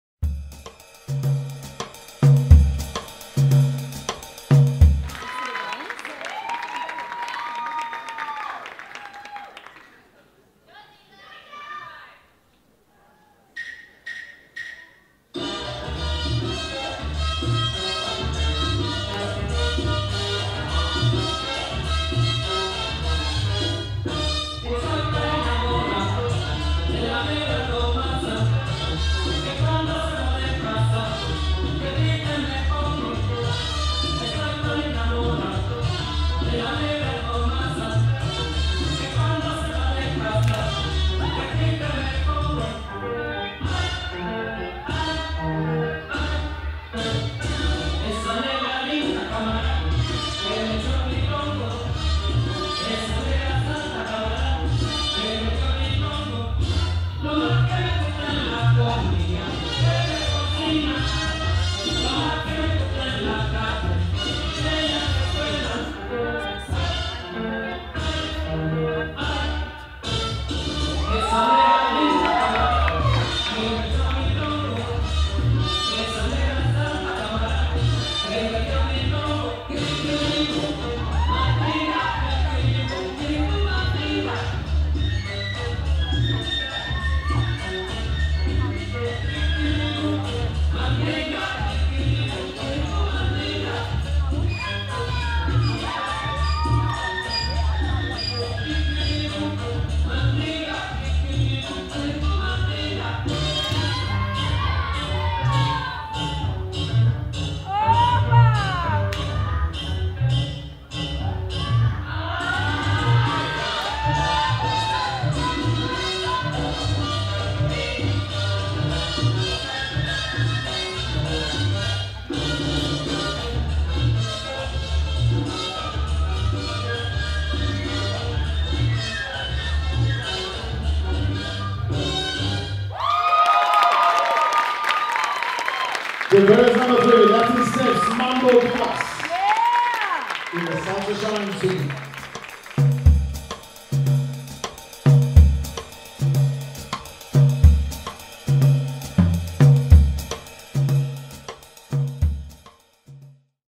Salsa Solo 2016 Amateur Salsa Shines Team Latin Steps Mambo Divas
Brisbane Latin Dance Festival
A Latin dance experience like no other!

World Salsa Solo
Australia's most prestigious national & international Latin dance competition!